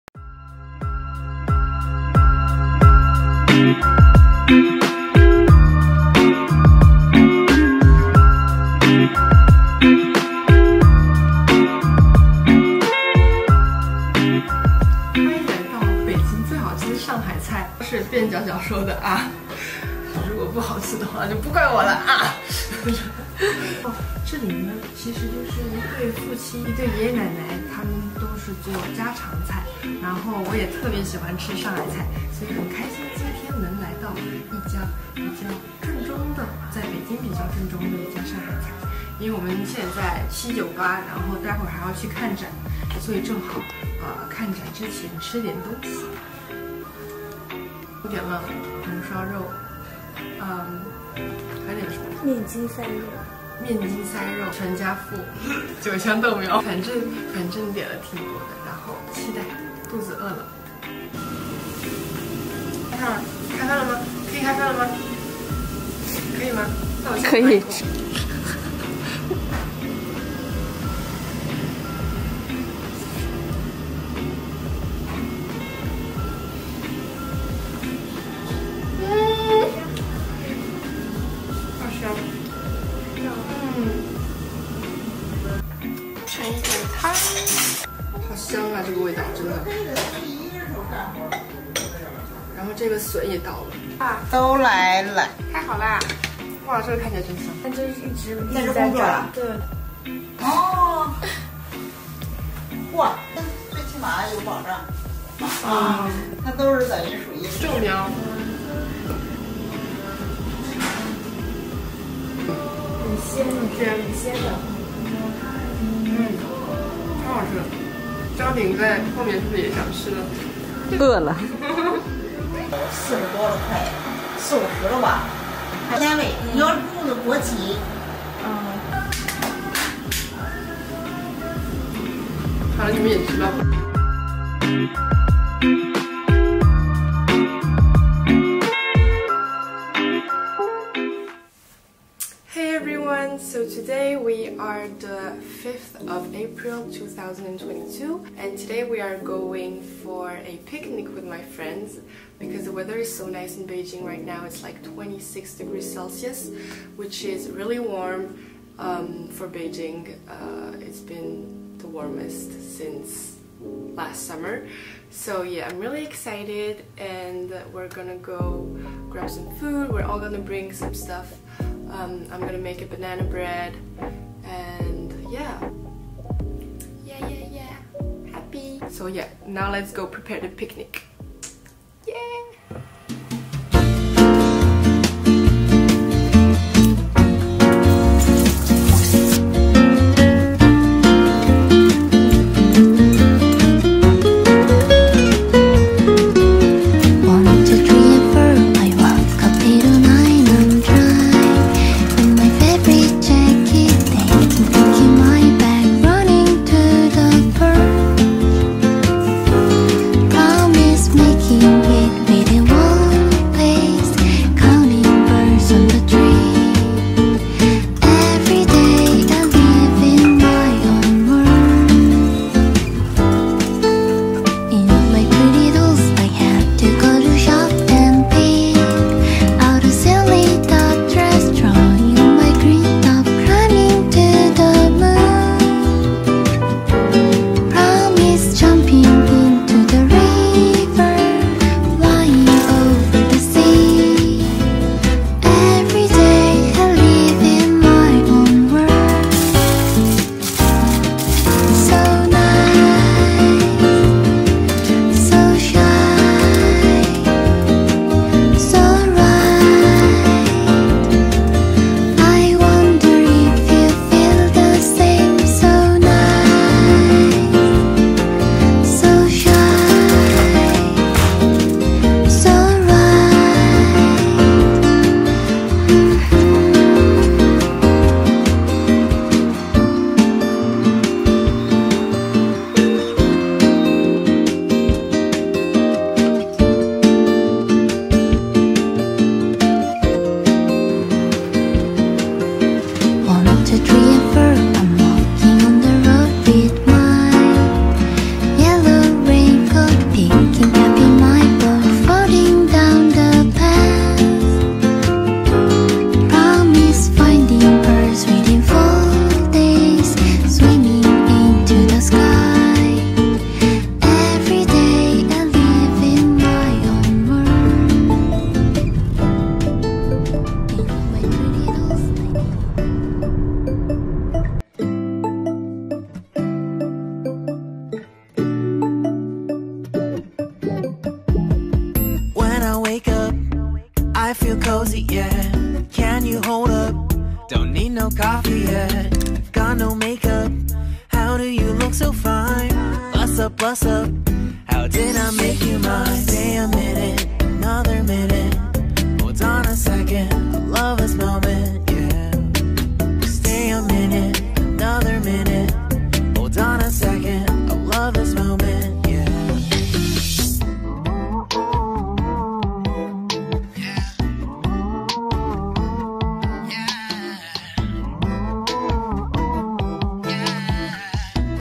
Shanghai cuisine, picnic, baking a banana bread... | Happy Foods Diary
Short vlog including some of the foods I ate this week!

Douyin: @estellechenyu